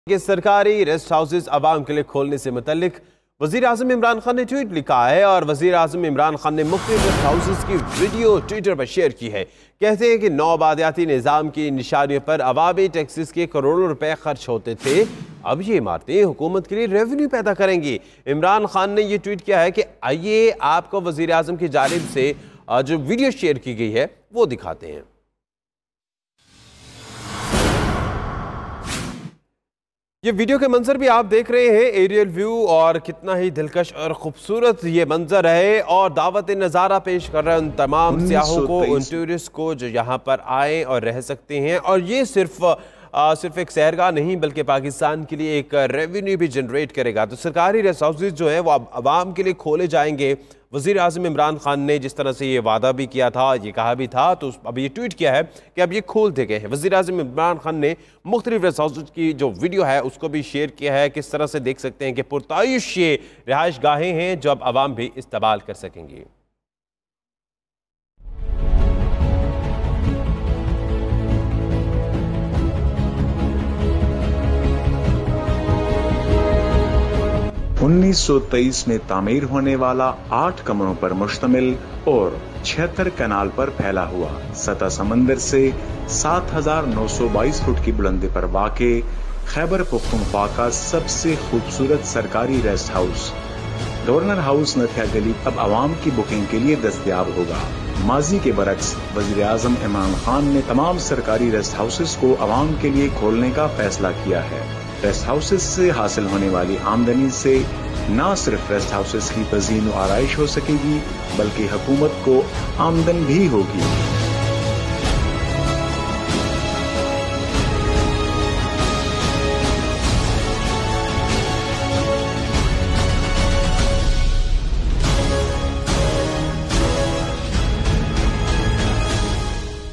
PM Imran announces to open Governor House in Nathiagali for public | 25 August 2019 | 92NewsHD
Watch: PM Imran announces to open Governor House in Nathiagali for public | 25 August 2019 | 92NewsHD